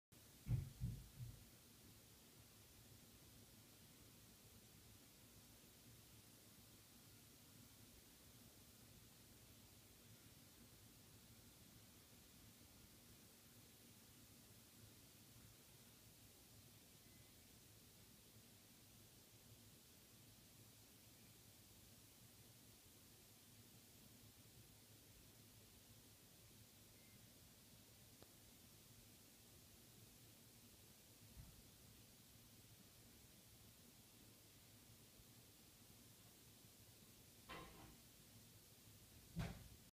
Jack staring at me 1
My cat Jack was staring at me without bilking his eyes for such a long... This video is only partial.